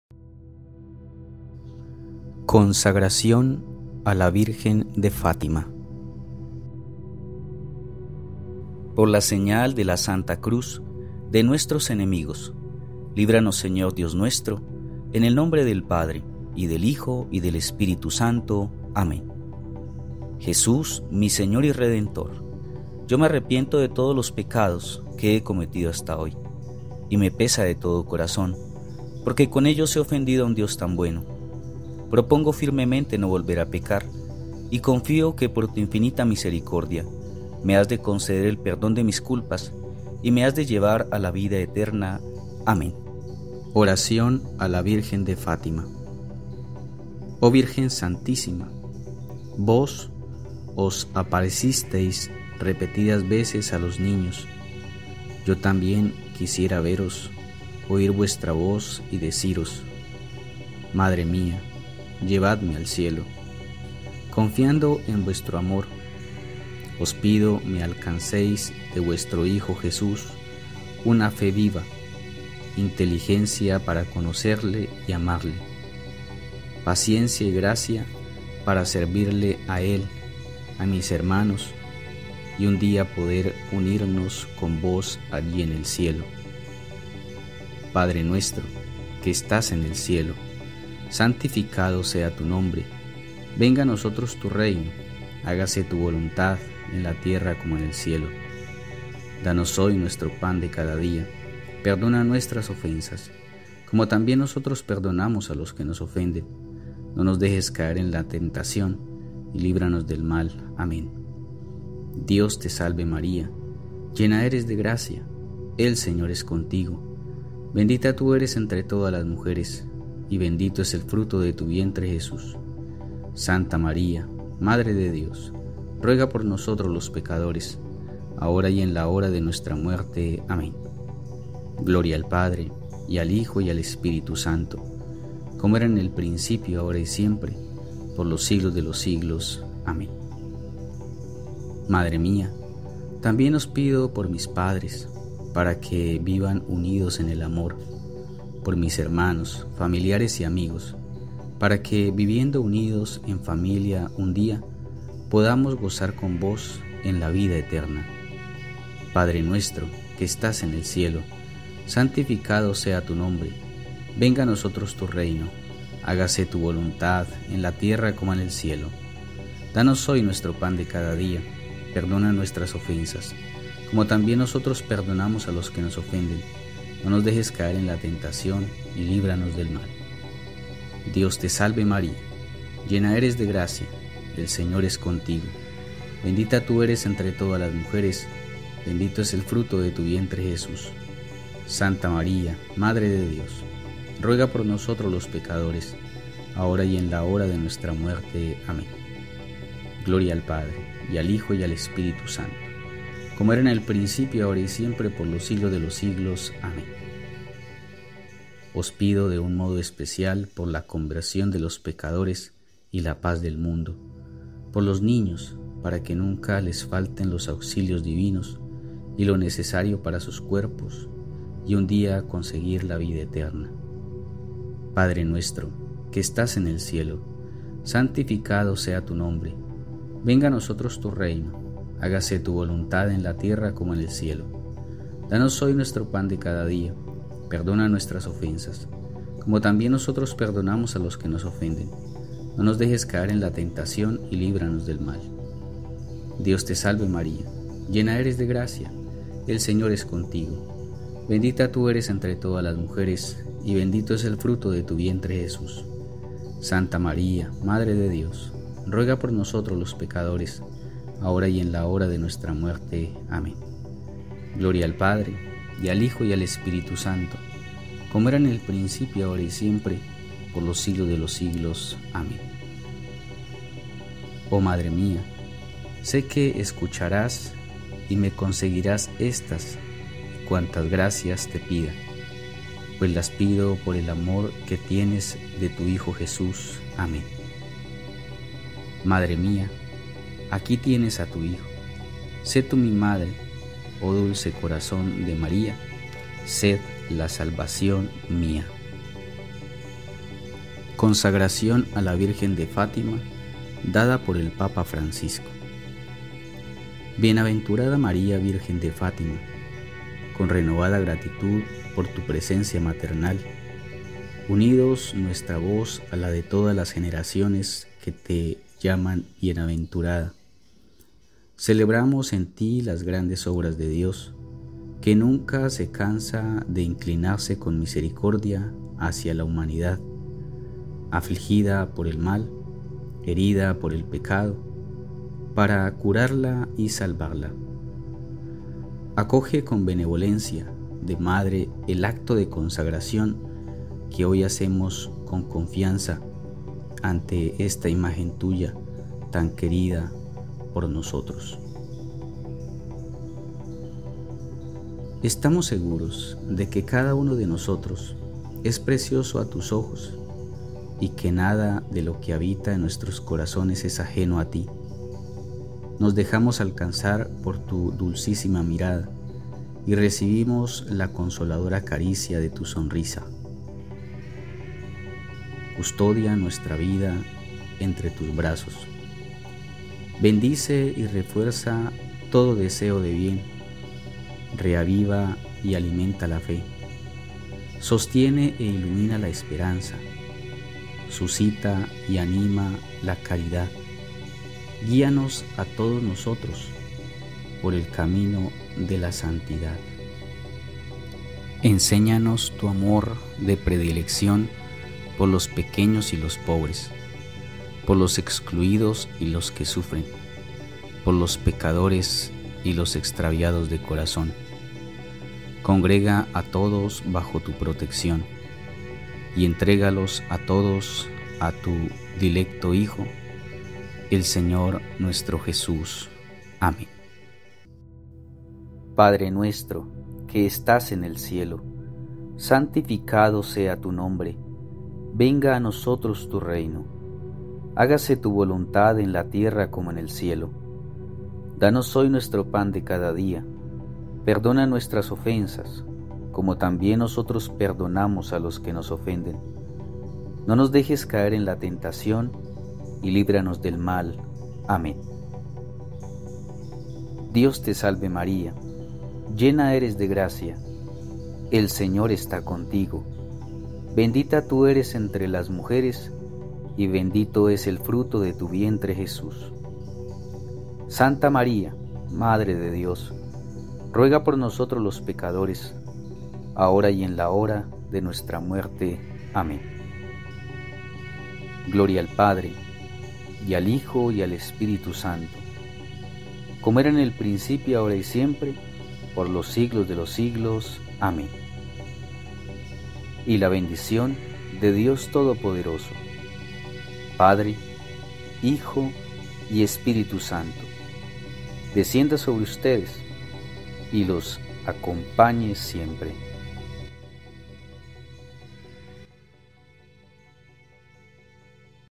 🙌🙌🙏CONSAGRACIÓN A LA VIRGEN DE FÁTIMA🙏🙏😇
Devoción a la Virgen de Fátima

Esta advocación mariana lleva desde 1917 muy presente en la Iglesia Católica. Tanto en el rezo del Rosario, como el lugar de peregrinación y en el fervor de fieles y papas. El día 13 de mayo celebramos a la Virgen de Fátima, conmemoración de su primera aparición en Cova da Iria, Portugal.
La mejor forma como nos puedes apoyar es 👇
✨ Comparte con amigos y familiares 👥
✨ Y déjanos tus comentarios 💬